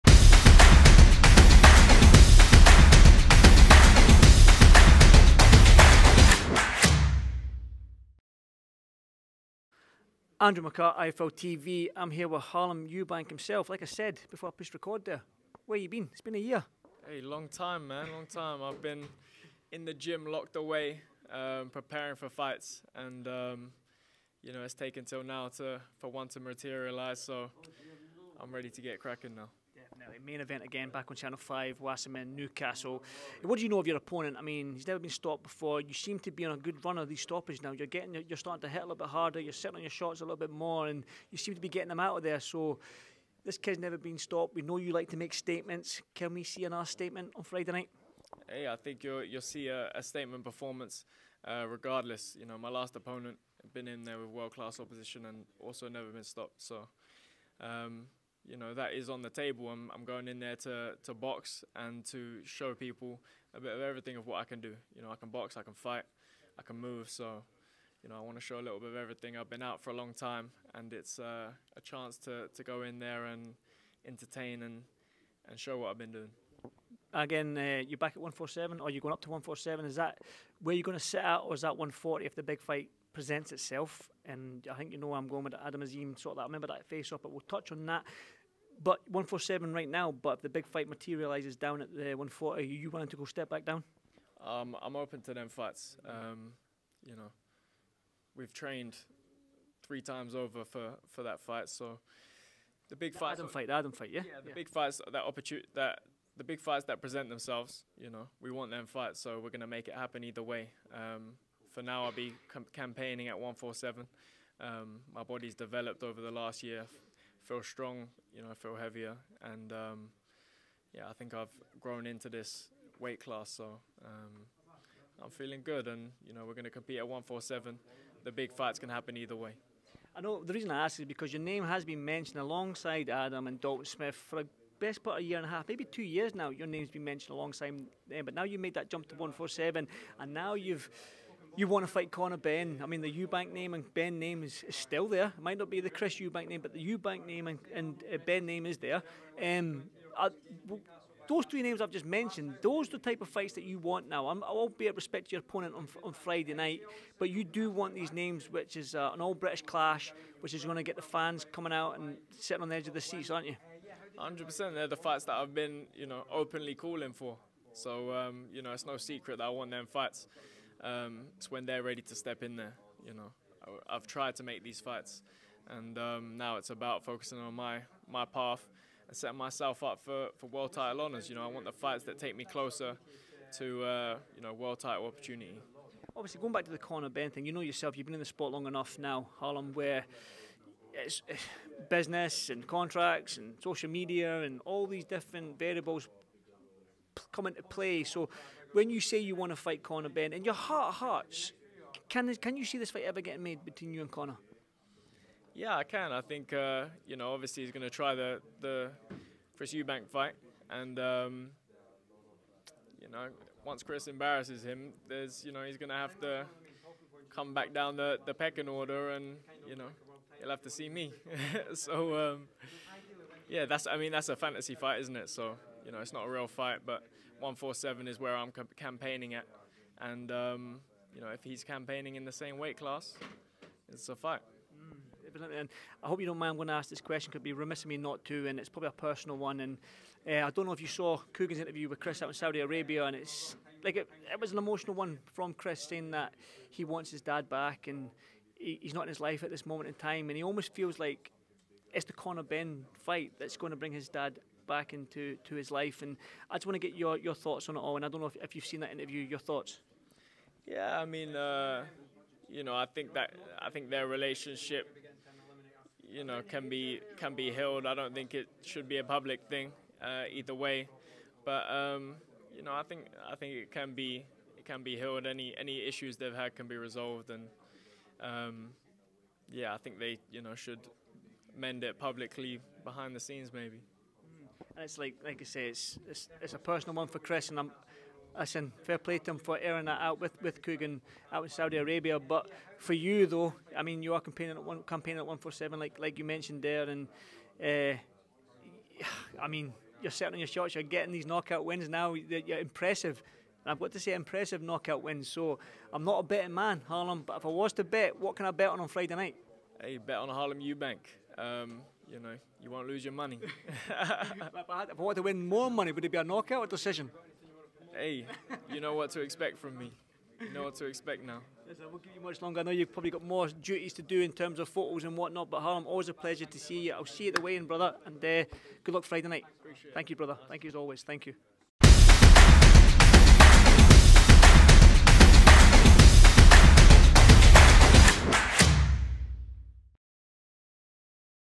'THEIR ISSUES CAN BE HEALED' - HARLEM EUBANK ON CHRIS EUBANK SR/JR SITUATION, TALKS CONOR BENN
iFLTV's Andrew McCart caught up with Harlem Eubank ahead of his main event fight on Channel 5, we discuss Chris Eubank Sr & Jr situation & potential Conor Benn fight.